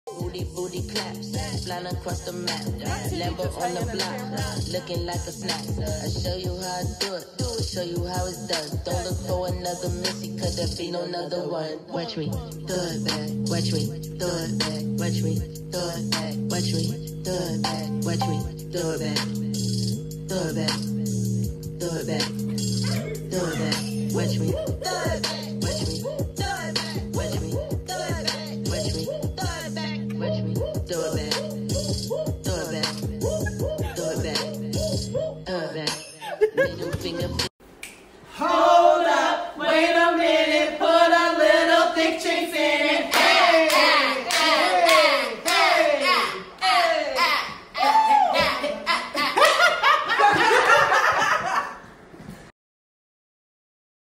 Behind the scenes with the Thick Chixx
Do you want to know what happens in our practices? Take a behind the scenes look at what really goes on.